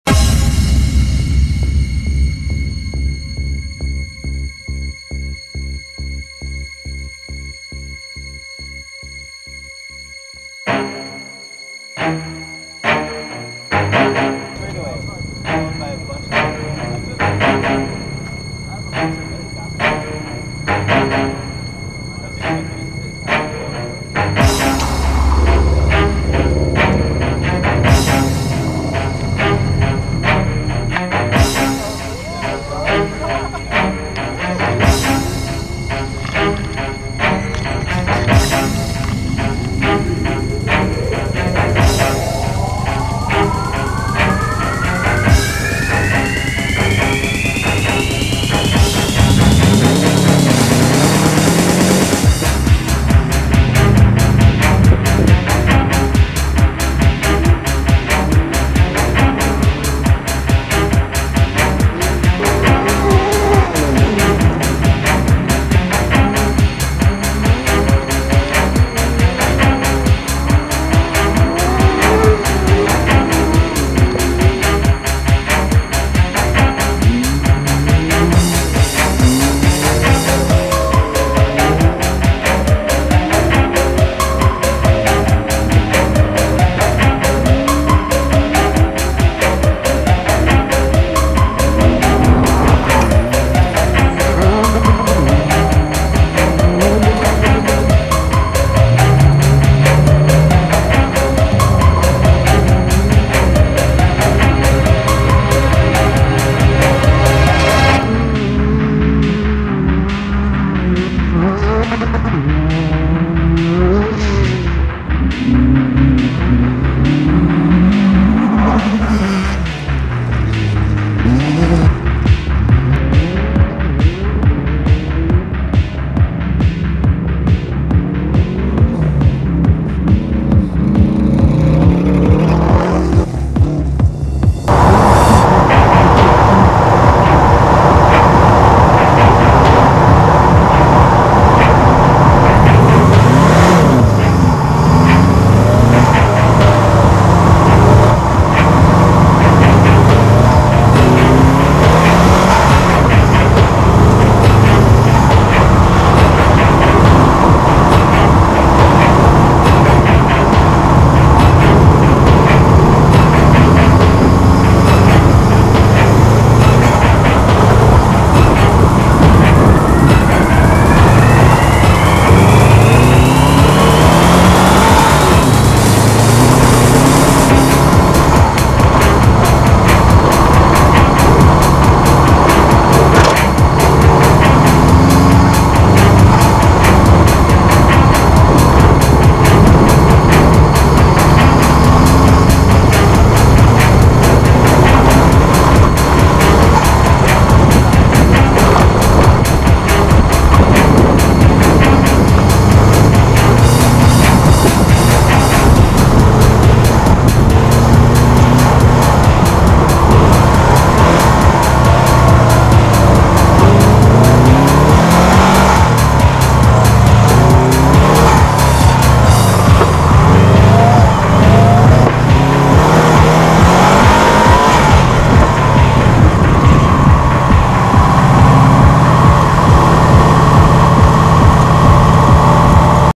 2016 CalClub RallyX Season Opener at Glen Helen Raceway
Videos of practice day, February 13 2016, at the SCCA CalClub RallyX season opener at Glen Helen Raceway in San Bernardino, CA. Last video is of my fastest run on Race Day. Hope you enjoy